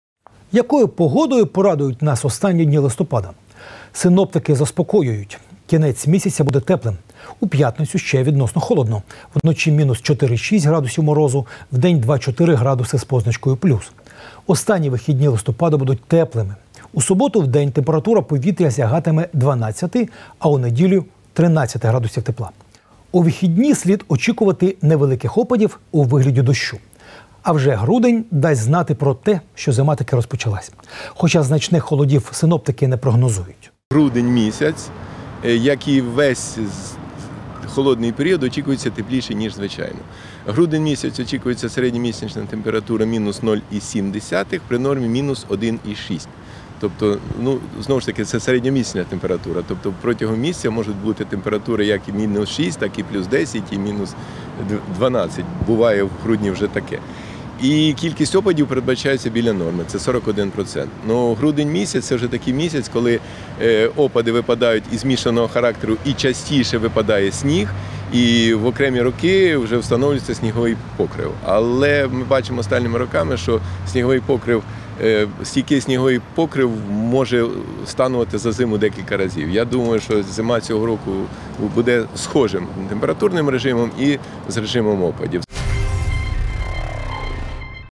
Пгода на листопад-грудень
Якою погодою порадують нас останні дні листопада? Синоптики заспокоюють: кінець місяця  буде теплим.  У п’ятницю ще відносно холодно - вночі мінус чотири - шість градусів морозу, вдень два-чотири градуси з позначкою. ПЛЮС. Останні вихідні листопада будуть теплими.  У суботу вдень температура повітря сягатиме дванадцяти, а у неділю - тринадцяти градусів тепла. У вихідні слід очікувати невеликих опадів у вигляді дощу.  А вже грудень дасть знати про те, що зима таки розпочалась. Хоча значних холодів синоптики не прогнозують.